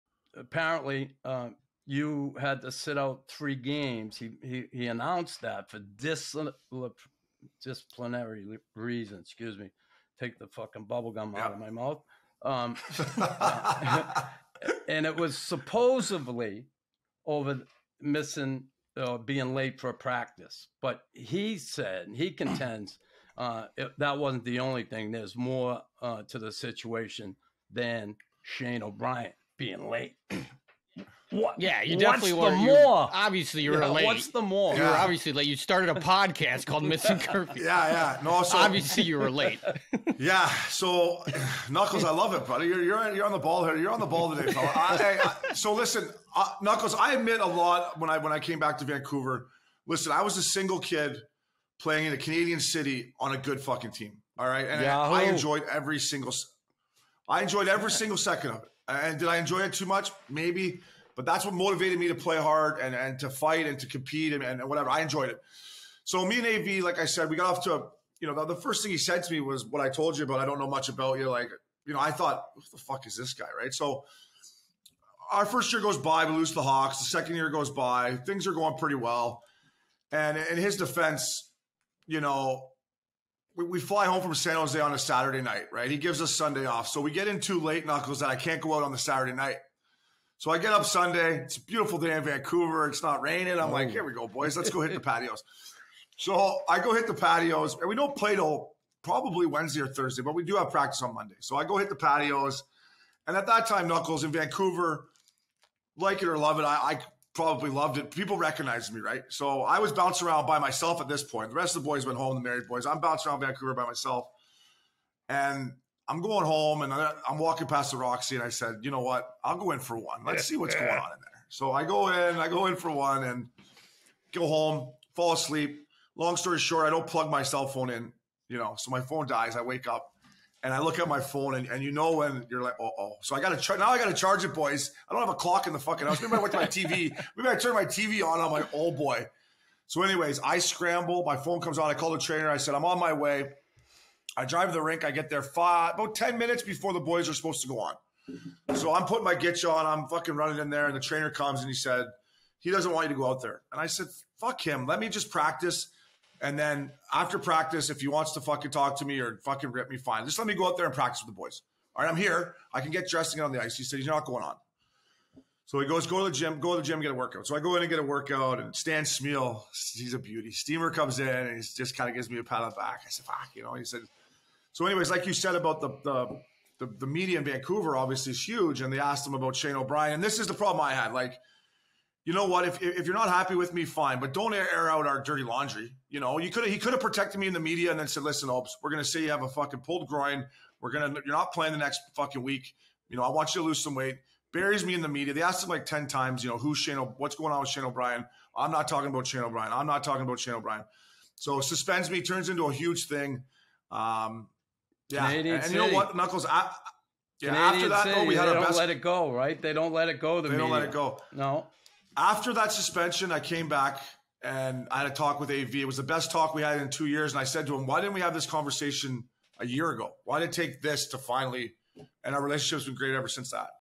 SHANE O'BRIEN MISSED CURFEW!
A sneak peak from our interview with Shane O'Brien, releasing on April 13, 2023!

🚶‍♂️GEAR & MERCHANDISE🚶‍♀️

📱SOCIAL MEDIA📱